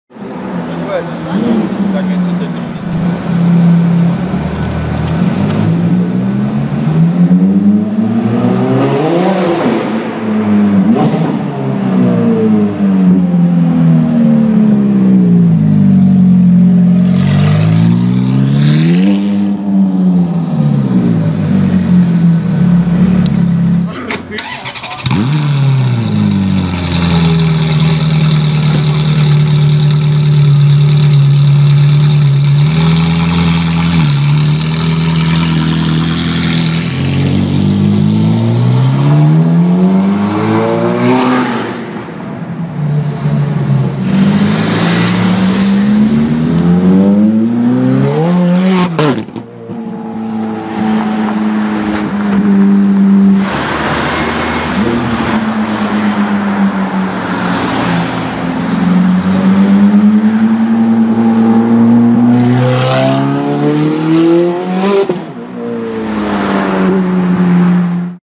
Ferrari 430 Scuderia w/ Capristo exhaust loud sounds!
Today I recorded a loud Ferrari F430 Scuderia with Capristo exhaust! The car is build by Luxury Costum Switzerland. In the video you can see some accelerations, flybys, a start up and loud downshifts. Lovely sound!
Camera: Canon Legria HF M506